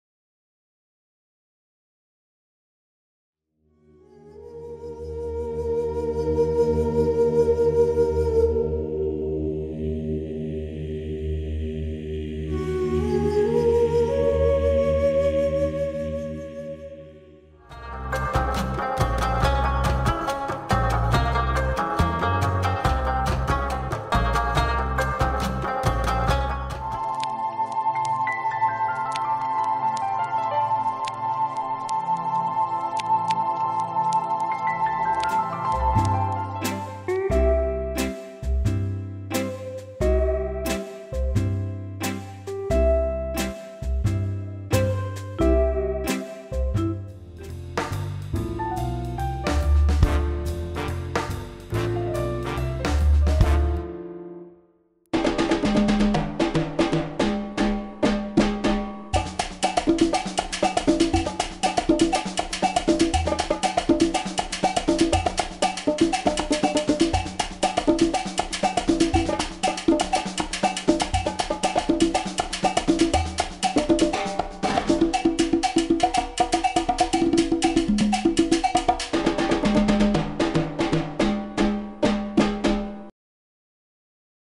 Aux 6 coins du monde /Stage vacances été 2024 - Labodanim - Mâcon
Un personnage parcours le monde…
Réalisation : Olivia 14 ans pendant une semaine de stage vacances
Technique : papier découpé